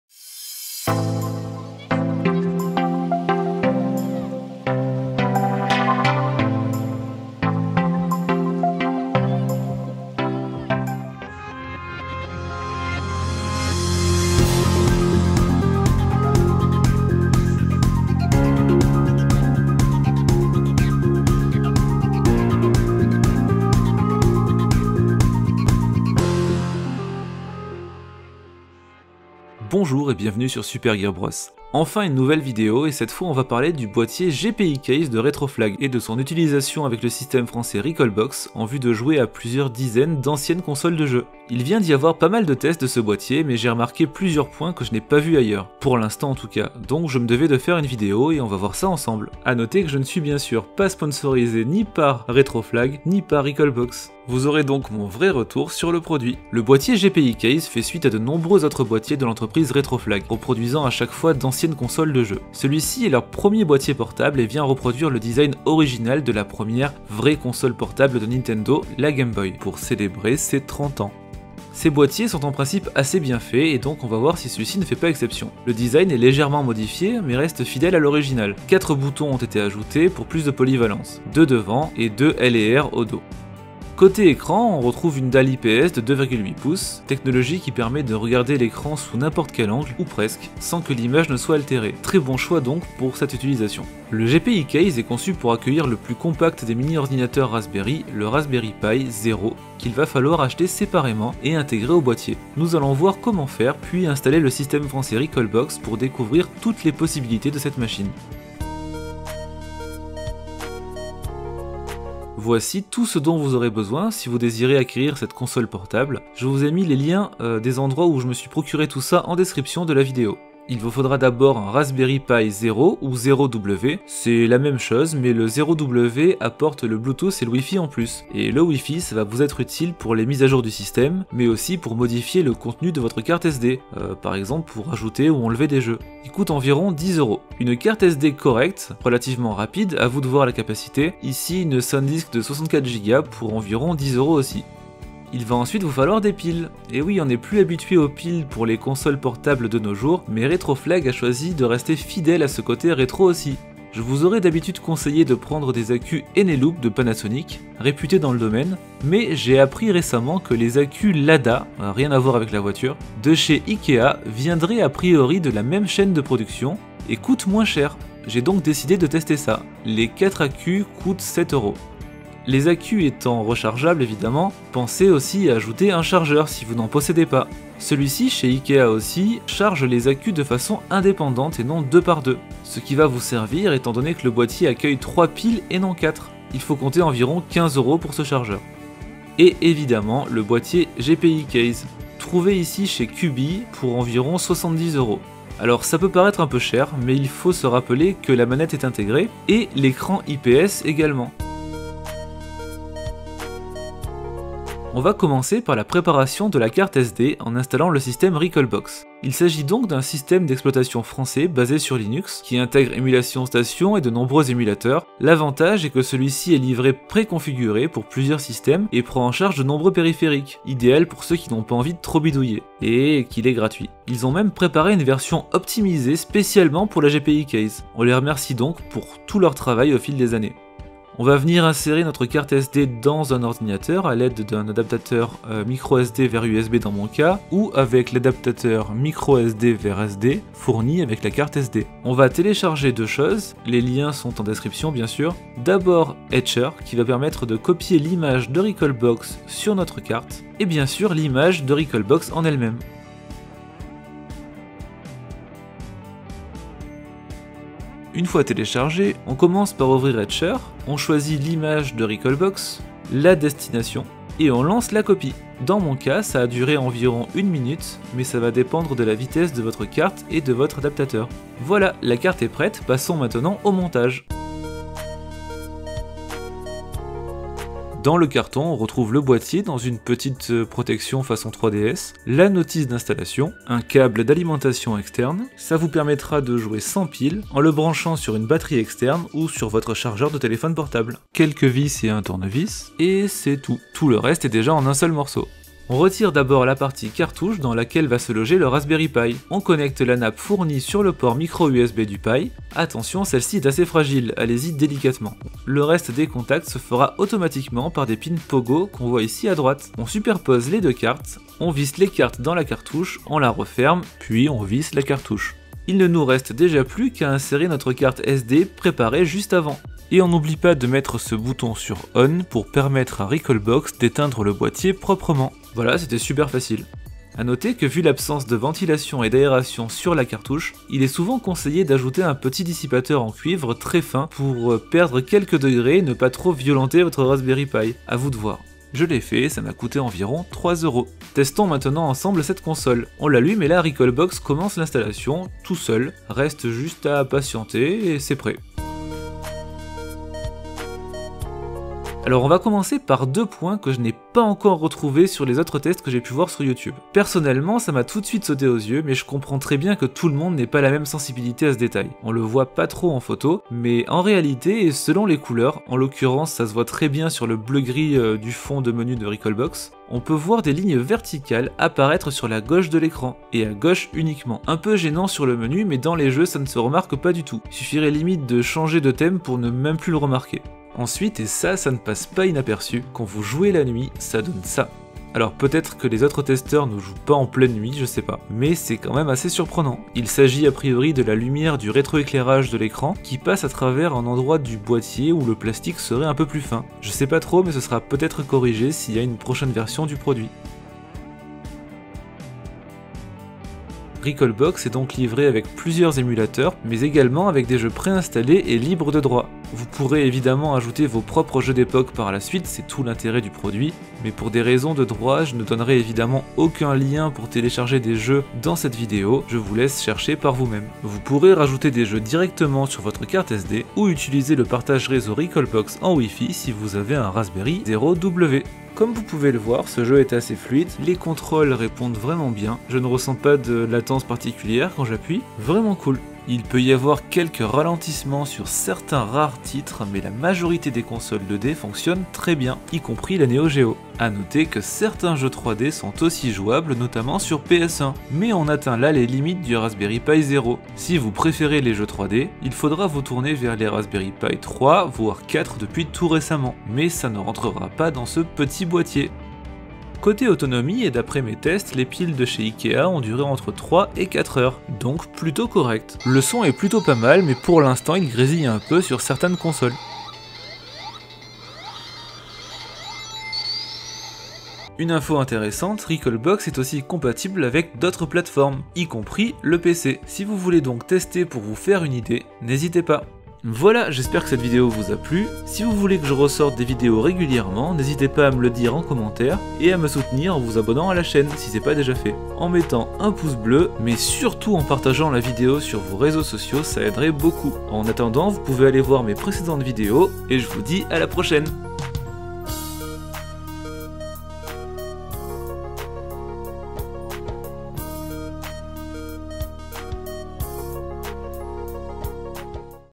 Retroflag GPi CASE et Recalbox - Découverte et explications
Accès rapide :

Intro - 0:00
Découverte - 0:27
Prérequis - 1:49
Préparation - 3:22
Montage - 4:35
Test - 5:49